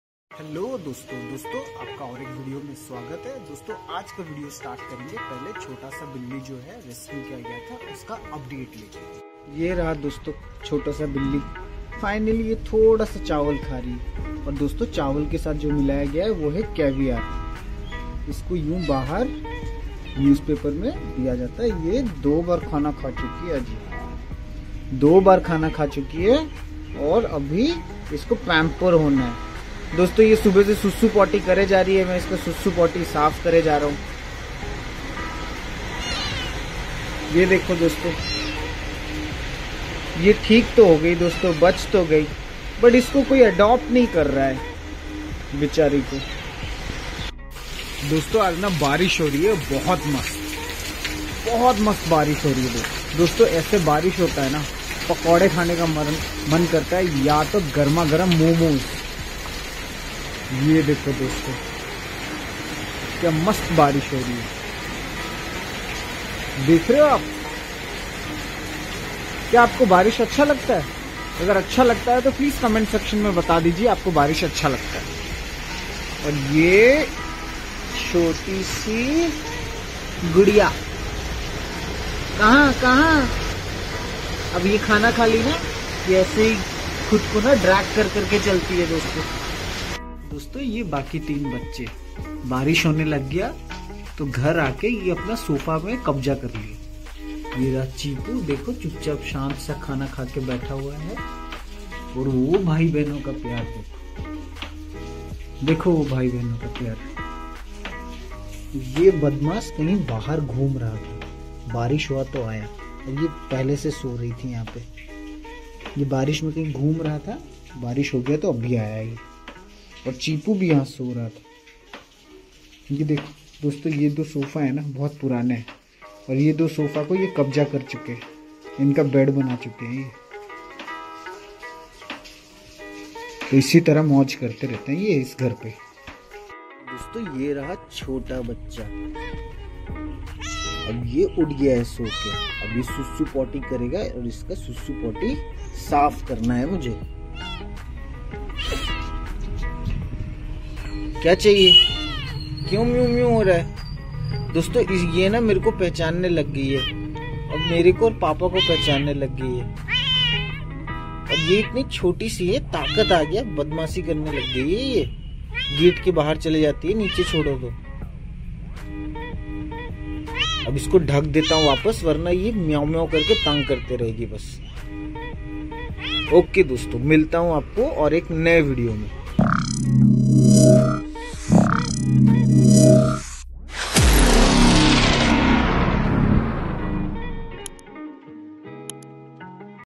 Rescued Kitten Life | Update of Cats
in this video i have shown you the little kitten which has been rescued and my house cats.